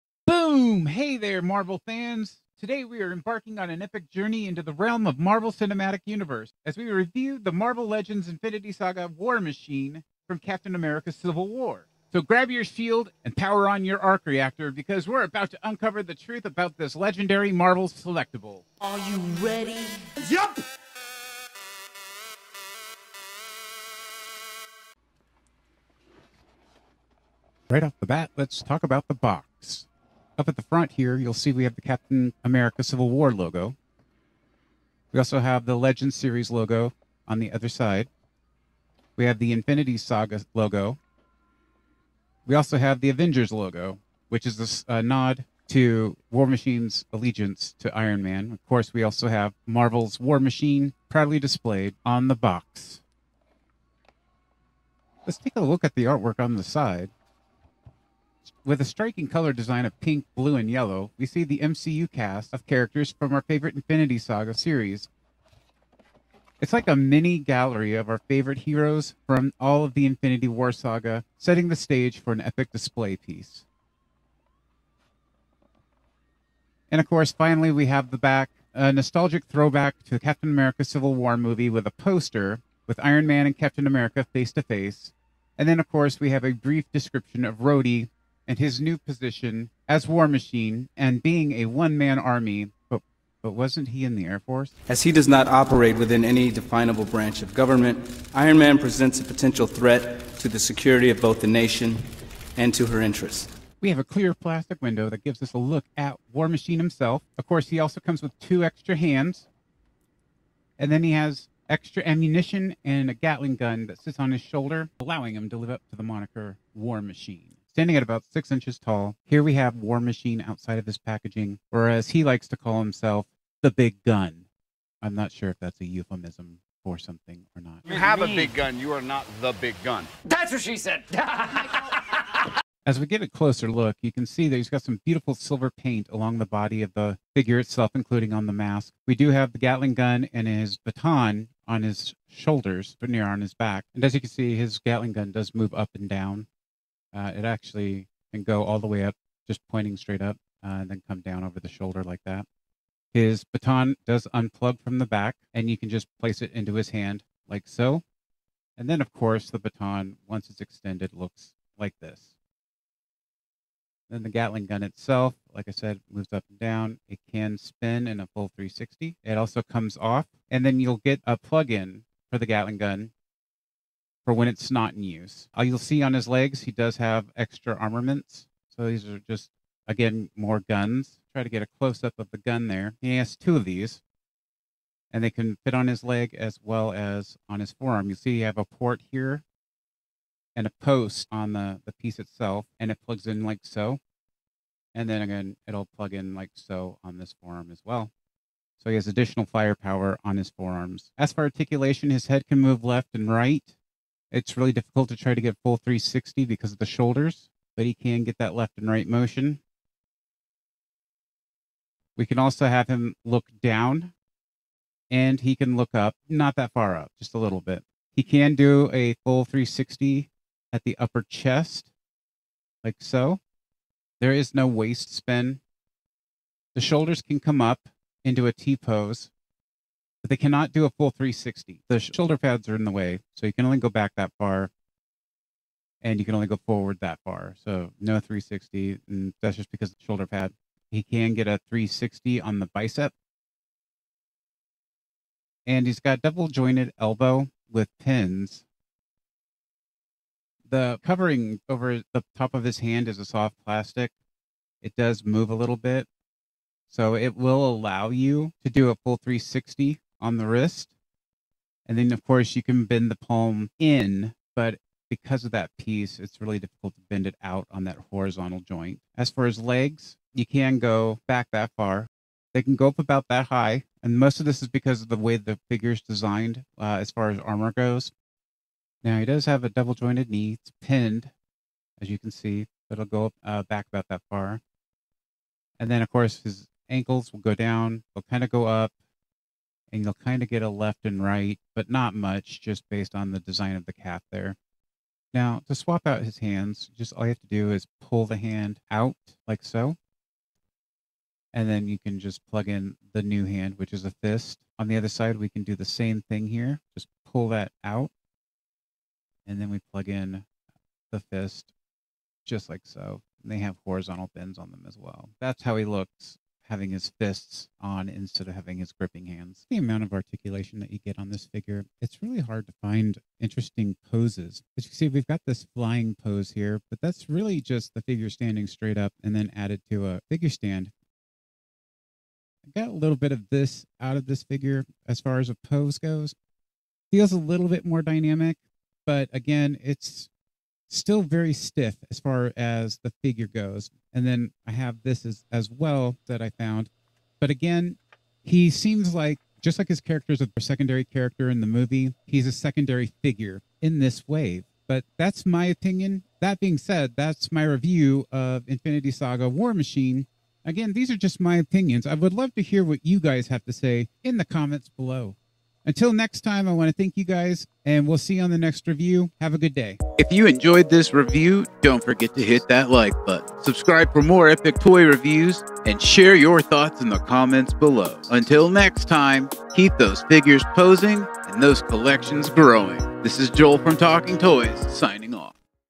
Boom! Unboxing the Marvel Legends War Machine!
Boom!  Hey there, fellow Marvel fans!
Today, we're embarking on an epic journey into the realm of the Marvel Cinematic Universe, as we review the Marvel Legends infinity Saga: War Machine from Captain America: Civil War!

Music created by Stevia Spehere - please support the artist by visiting the link